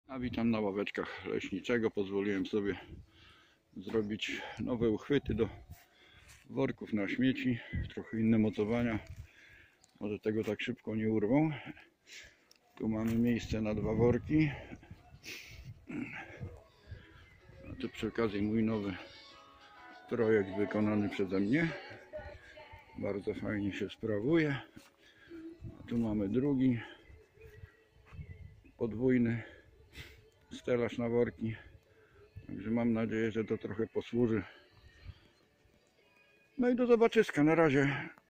worki, nowe stelaże
nowe mocowania worków na śmieci na ognisku